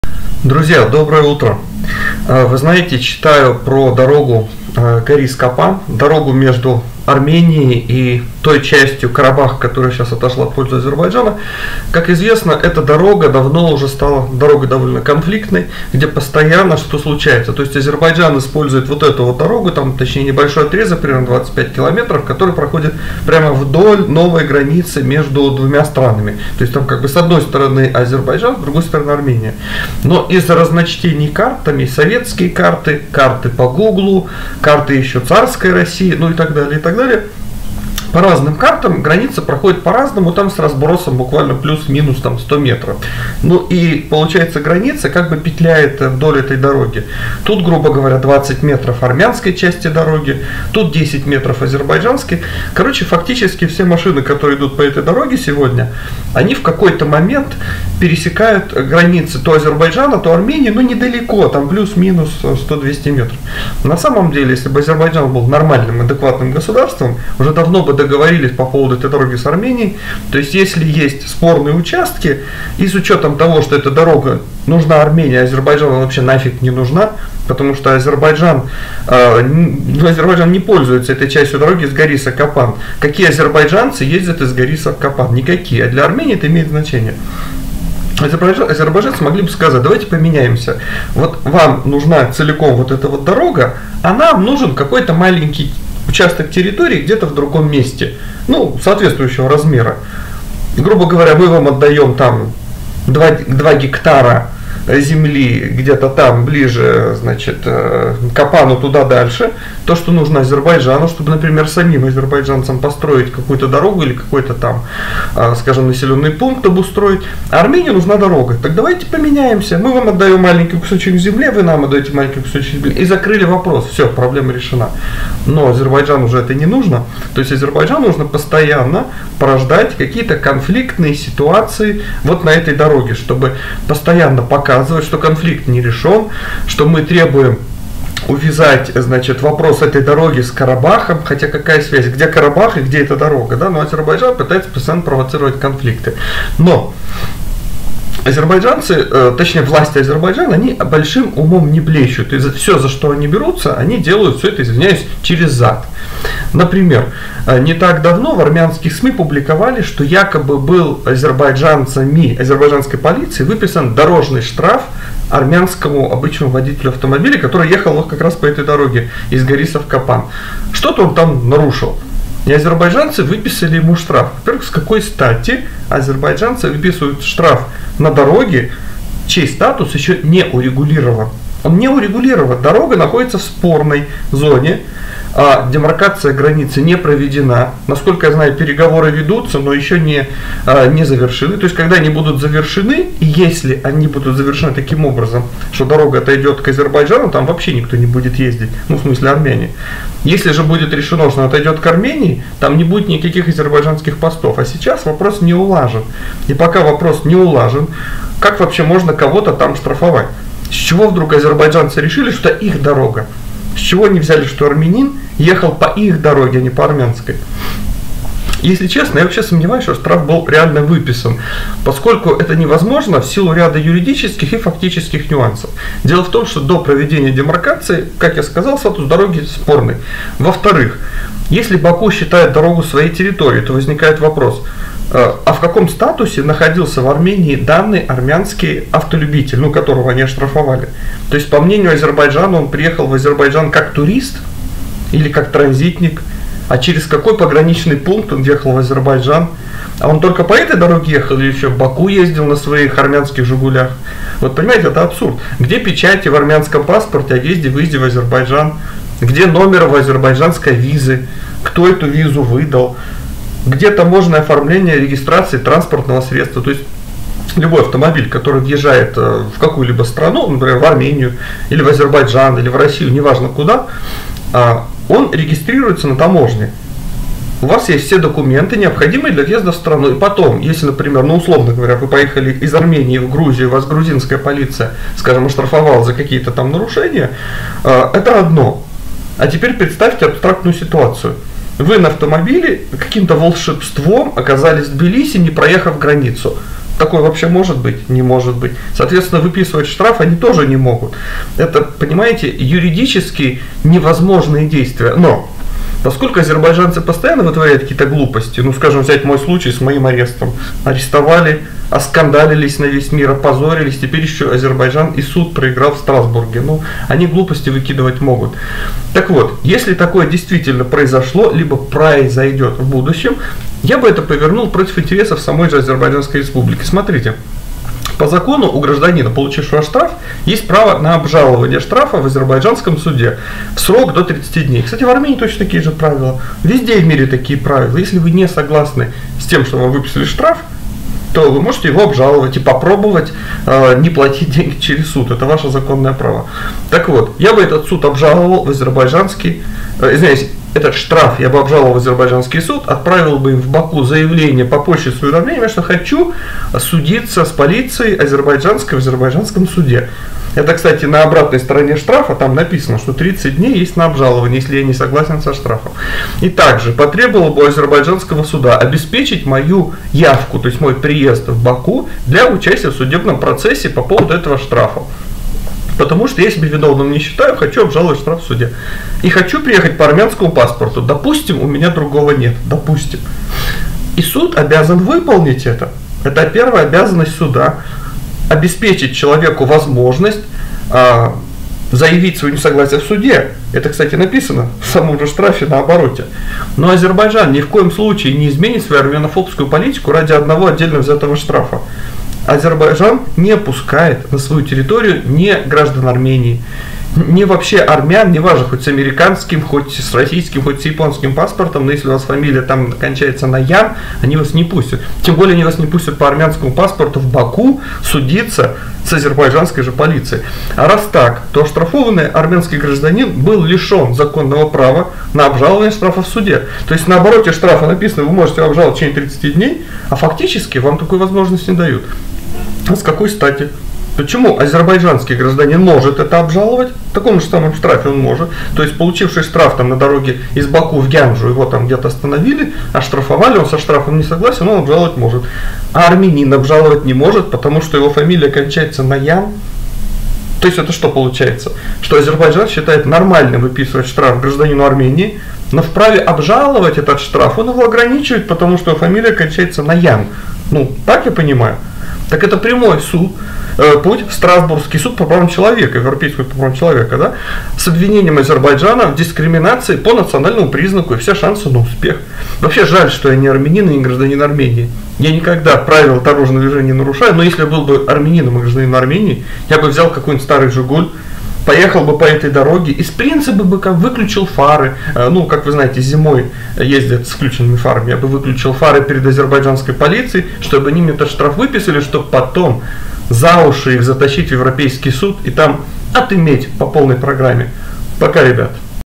АЗЕРБАЙДЖАНЦЫ ВЫПИСЫВАЮТ АРМЯНАМ ШТРАФЫ?
К вопросу о штрафе, якобы выписанном армянскому гражданину на автодороге Горис - Капан.